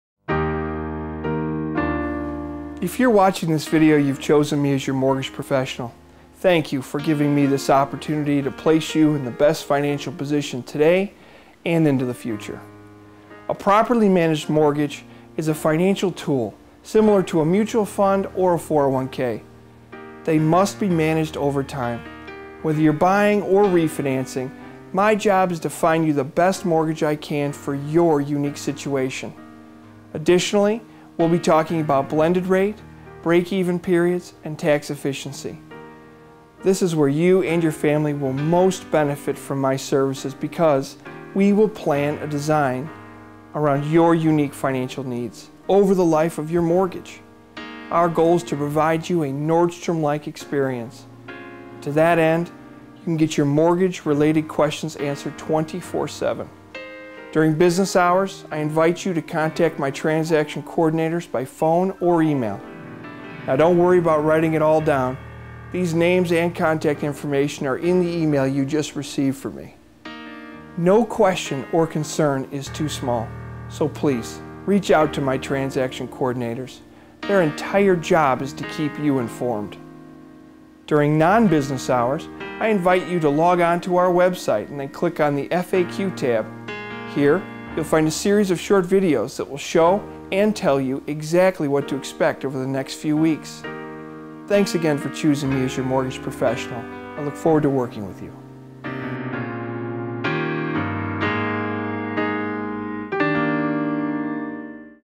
Welcome to Mortgage Capital Group in Crystal Lake, Illinois
Congratulations, if you've been directed to this video you've probably already decided that Eric Schroeder and his Mortgage Capital Group associates are the right people to find the perfect mortgage for you and your family. Mortgage Capital Group's approach to business is simple; it is relationship-based versus transaction based.